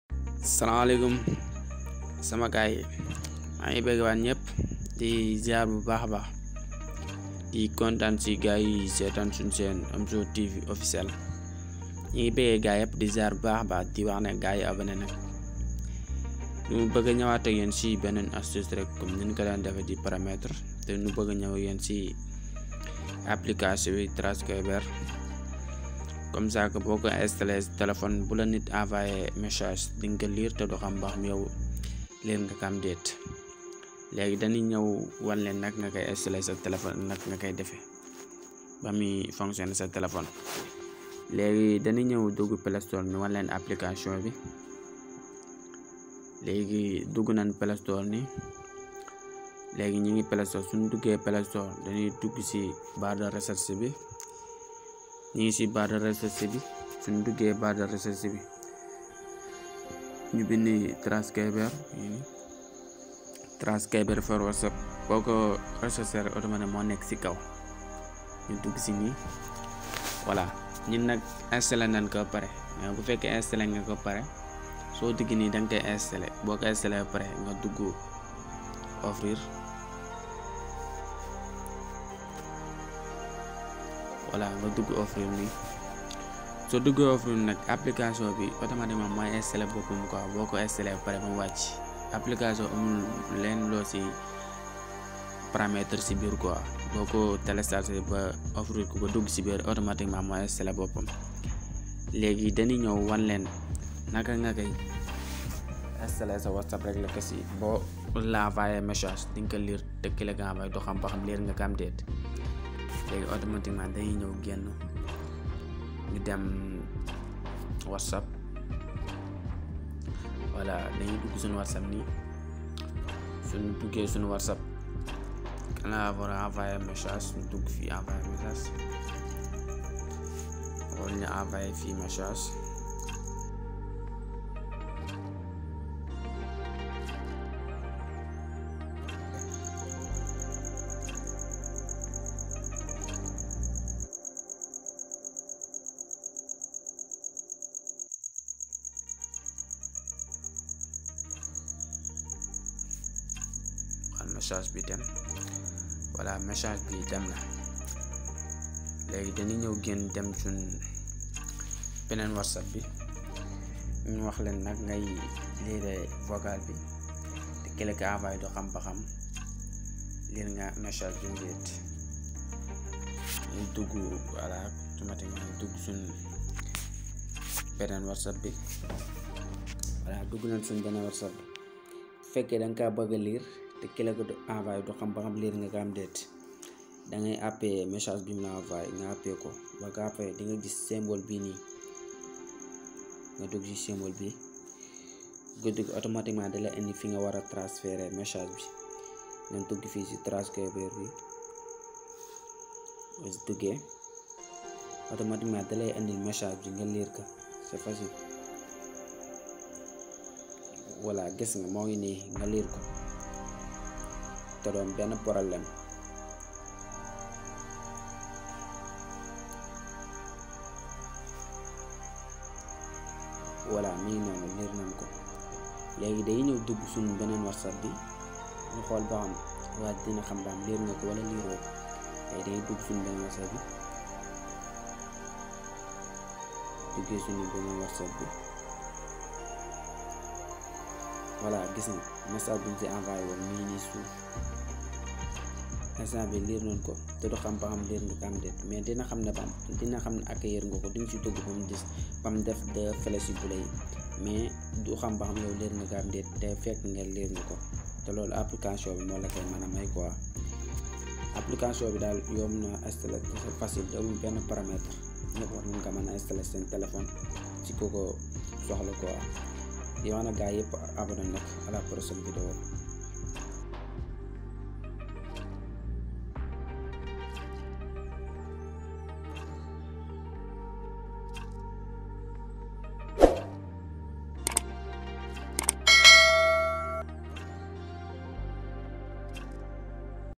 comment_lire une message vocal santé WhatsApp
comment lire en messages vocal WhatsApp sans confirmation de lecture

ngana Tv

Sall communication

Omzo Tv Officiel

tuto en Wolof Sénégal